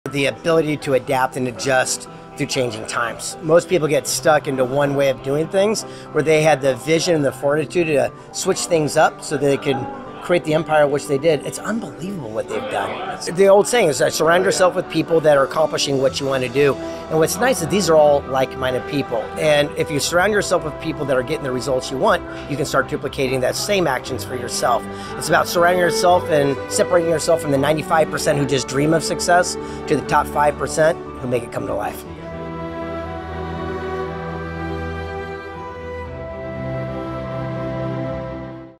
Greg Reid On Big Block Realty🏆🔥
We Caught up with Greg Reid at Winner's Circle and this is what he had to say about Big Block and achieving success.